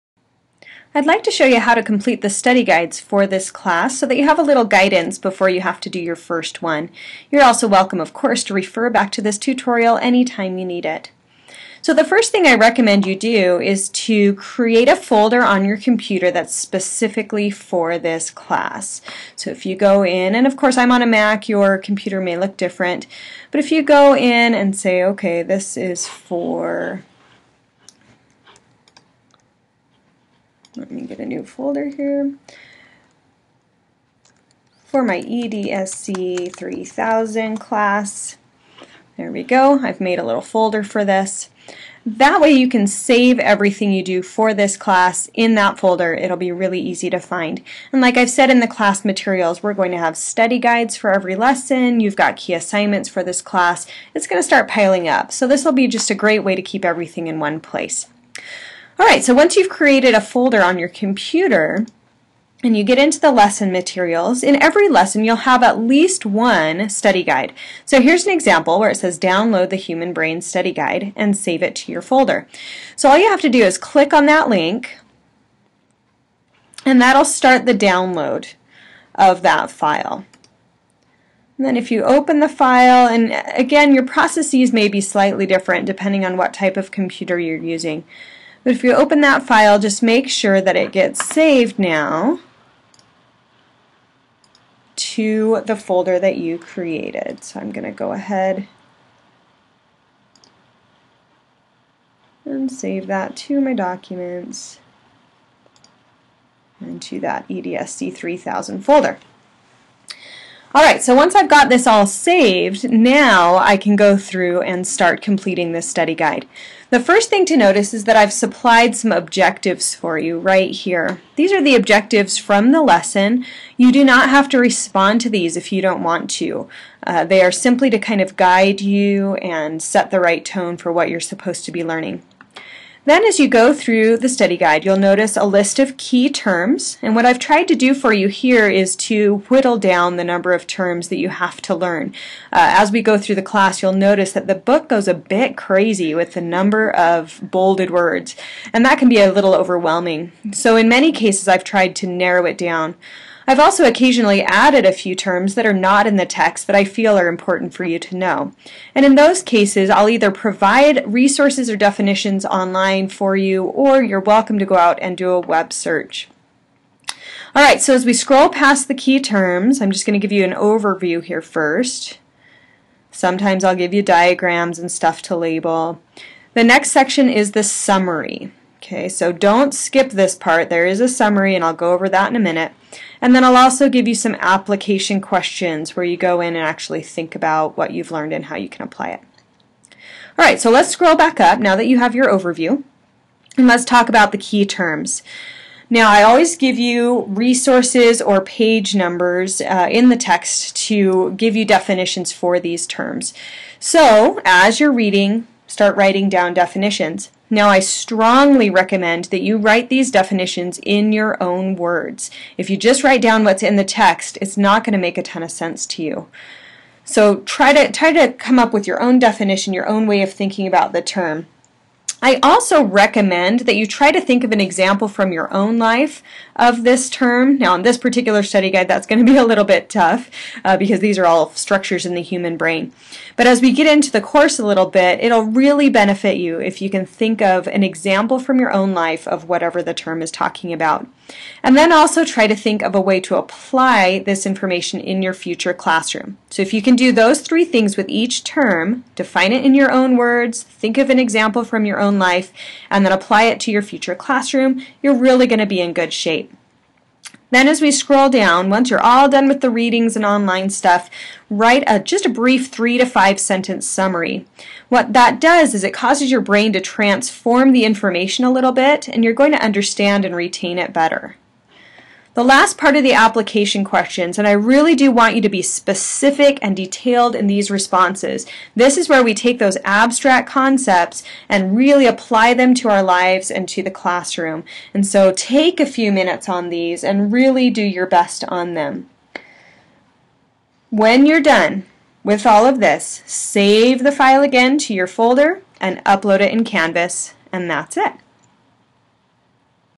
Study Guides Tutorial
A brief explanation of how to complete the study guides for EDSC 3000 at Utah Valley University